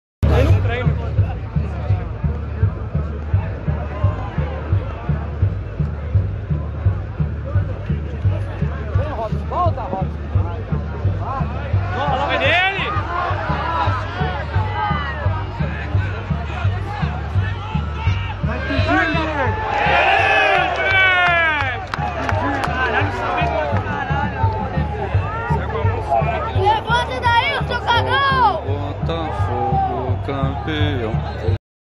Camapnha para converter meu filho santista num feliz rubro negro.mp4
Camapnha para converter meu filho santista num feliz rubro negro. Valeu de tudo em 2009, até ir à Vila Belmiro, ficar no meio da torcida santista e secar o peixe!
0 x 0 feio, com direito a chuva e tudo, mas o plano ia bem...